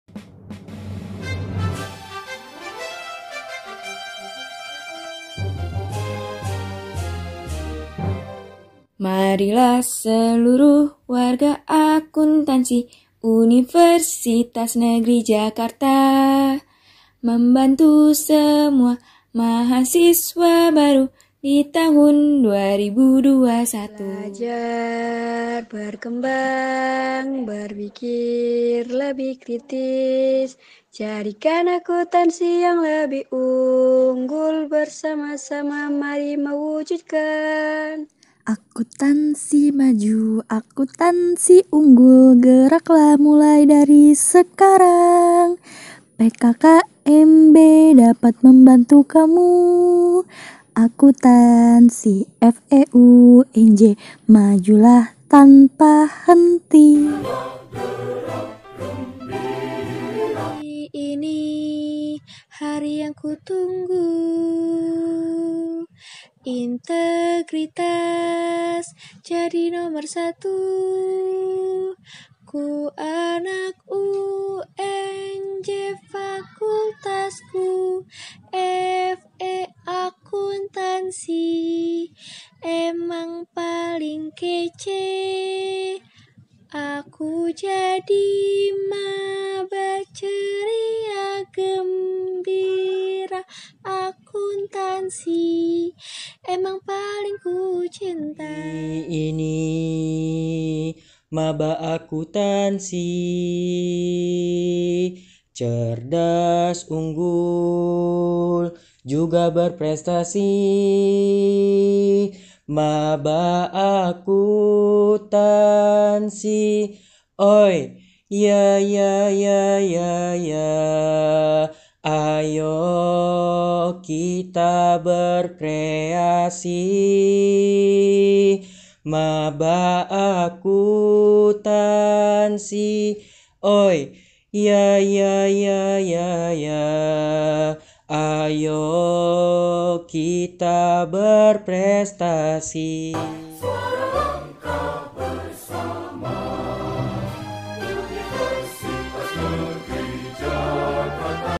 YEL-YEL PKKMB AKUNTANSI 2021
Hai, Sahabat Akuntansi!

Kamu Mahasiswa Baru Akuntansi FE UNJ 2021? Yuk, hafalkan yel-yel ini agar nantinya PKKMB Akuntansi makin meriah dan seru lohh.
Sampai bertemu sahabat!🔥